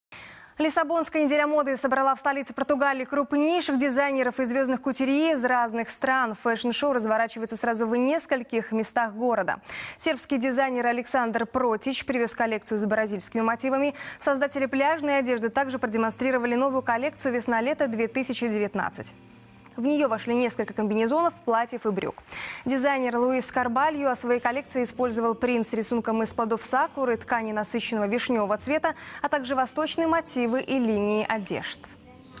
Лиссабонская неделя моды собрала крупнейших дизайнеров и звёздных кутюрье
Лиссабонская неделя моды собрала крупнейших дизайнеров и звёздных кутюрье из разных стран.

Фешн-шоу разворачивается сразу в нескольких местах города. Создатели пляжной одежды также продемонстрировали новую коллекцию. В нее вошли несколько комбинезонов, платьев и брюк.